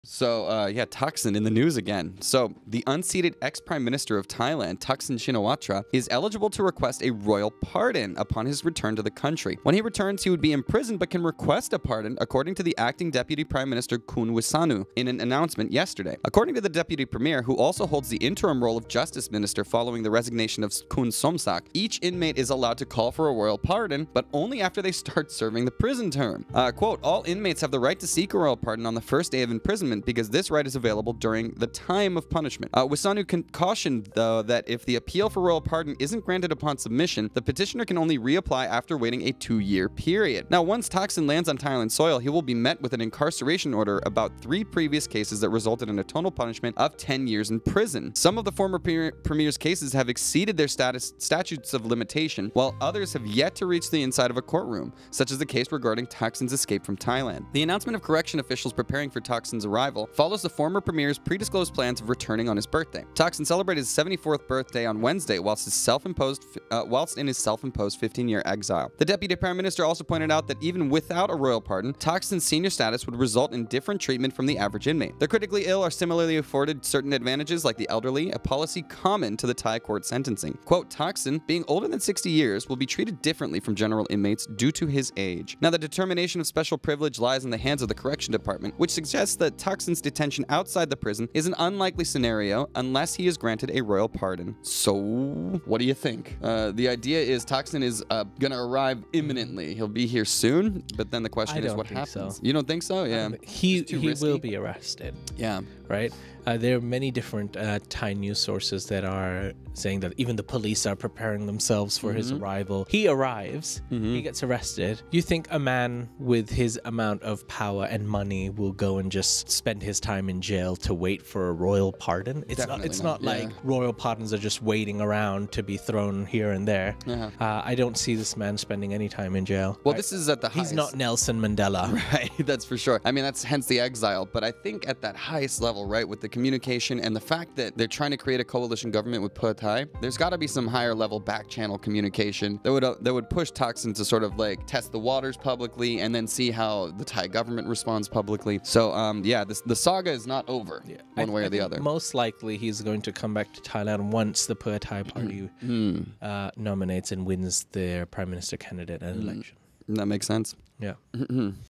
Thailand News | Royal pardon for ex-PM Thaksin?
The unseated ex-Prime Minister of Thailand, Thaksin Shinawatra, is eligible to request a royal pardon upon his return to the country. When he returns, he would be imprisoned but can request a pardon, according to the acting Deputy Prime Minister Wissanu Krea-Ngam in an announcement yesterday.

According to the deputy premier, who also holds the interim role of justice minister following the resignation of Somsak Thepsuthin in March, each inmate is allowed to call for a royal pardon, but only after they start serving their prison term.

“All inmates have the right to seek a royal pardon on the first day of imprisonment because this right is available during the time of punishment.”